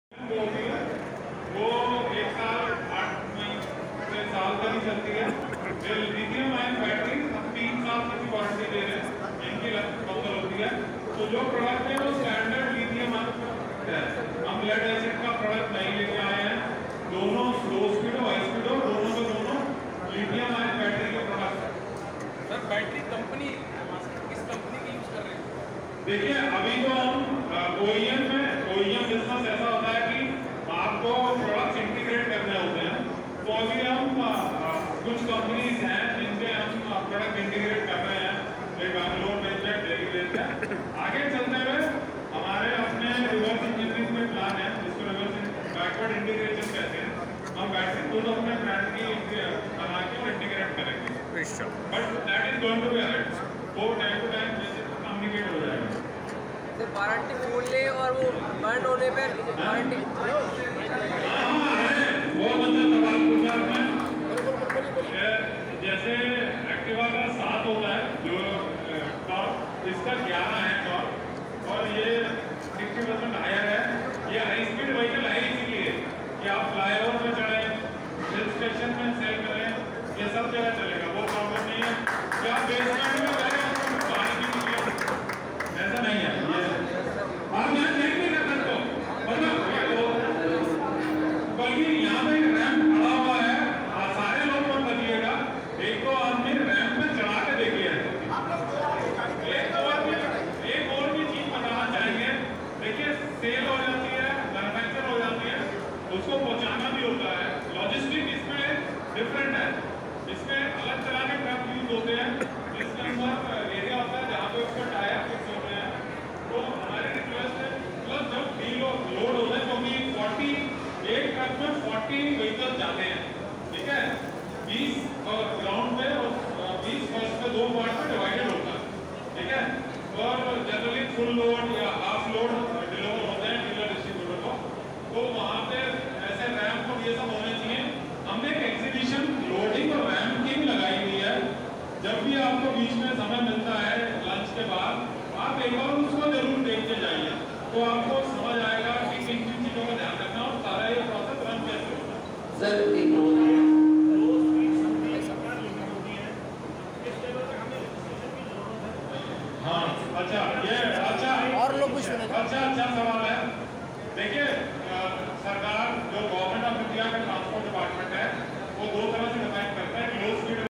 Information given by Wonder Ride Seniors officials at plant meeting , video by Dr. Pradip Saxena
Video from Dr.Pradip Saxena